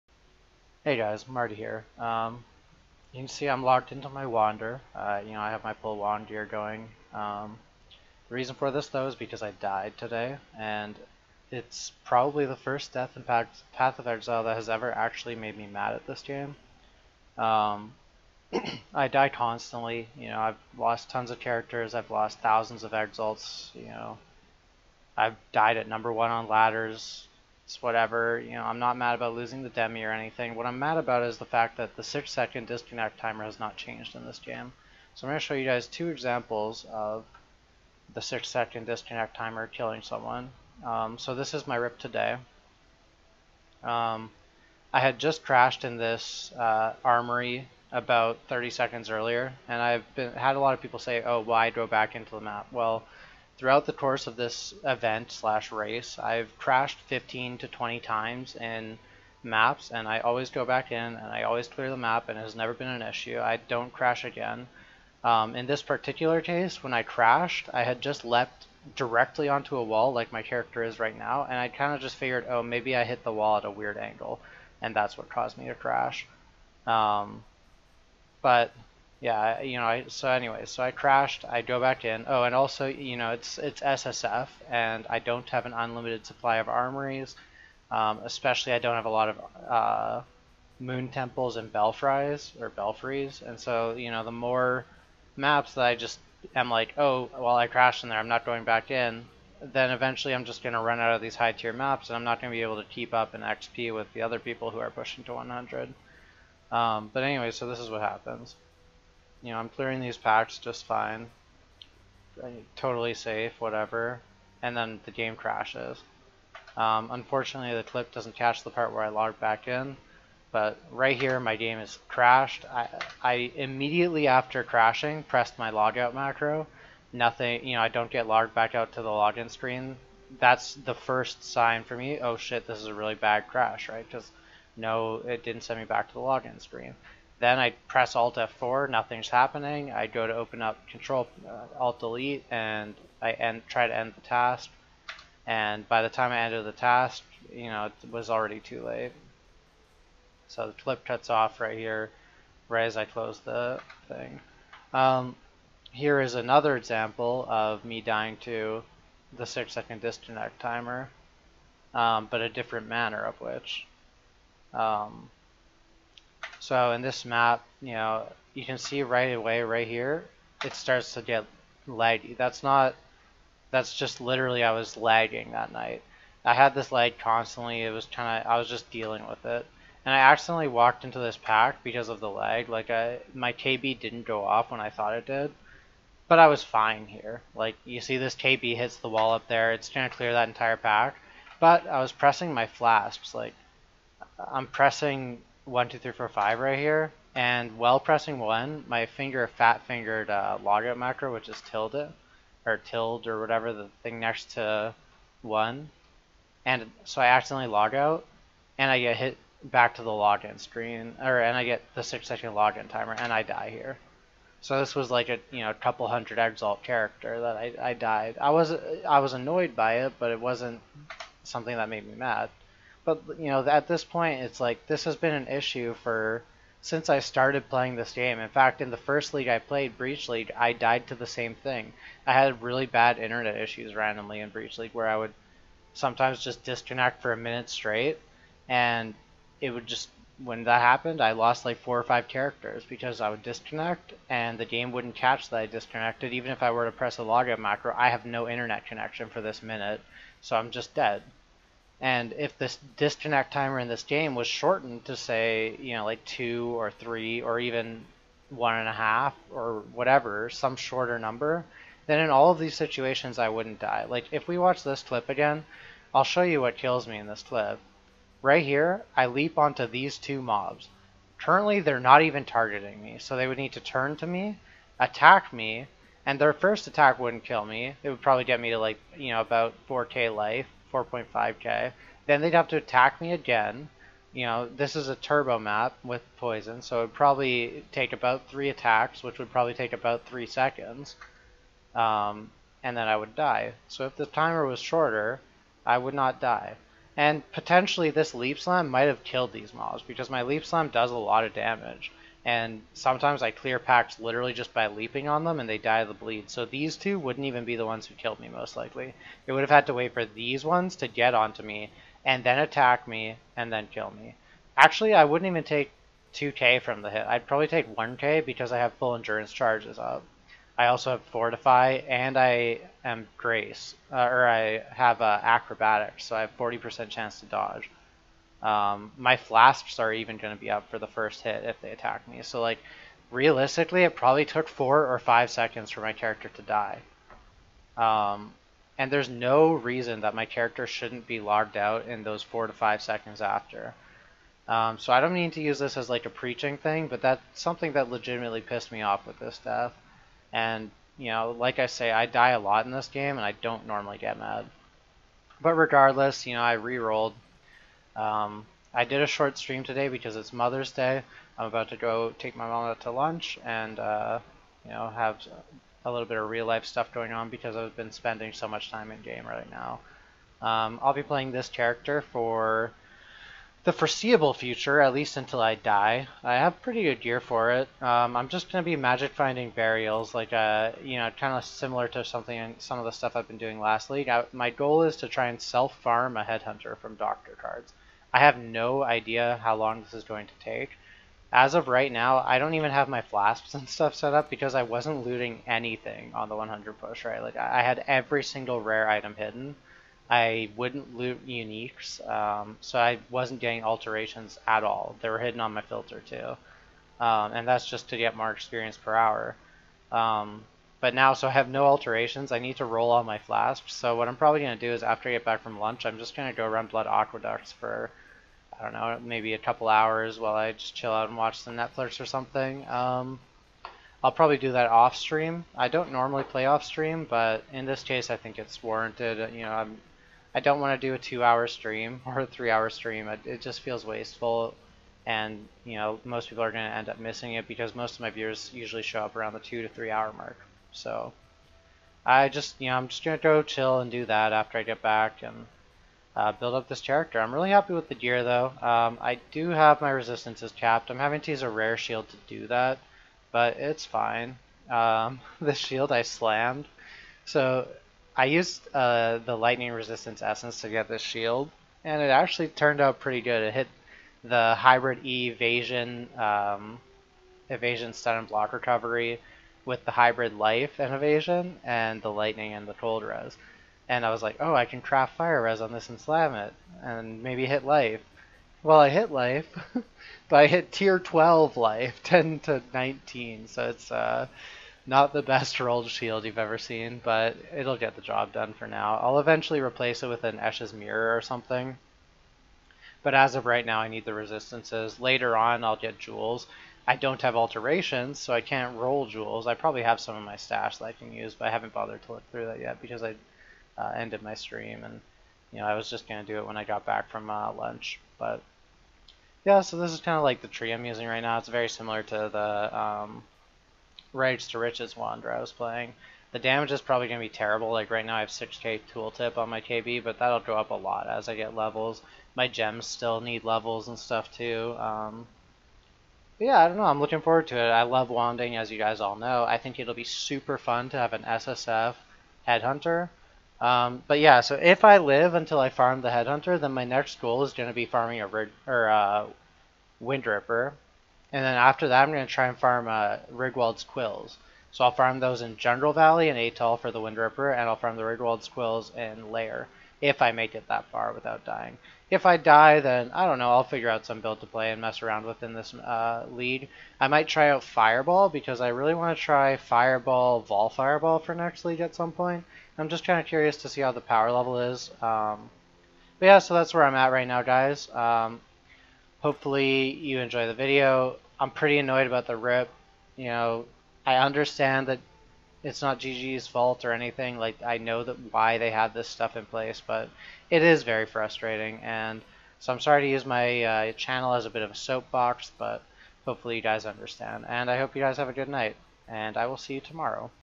May Flashback Update - Marty Down
This is the first death I've ever had that has made me mad, I'm not normally a rager and I apologize for ranting about it but I do think this is a problem with the game that needs to be looked into.

As you can see I've already levelled my wander because I absolutely love this game and a rip like this while disheartening won't stop me from playing such a great game. That being said nothing will make me happier than if I see the 3.4 patch notes including some change to the dc timer.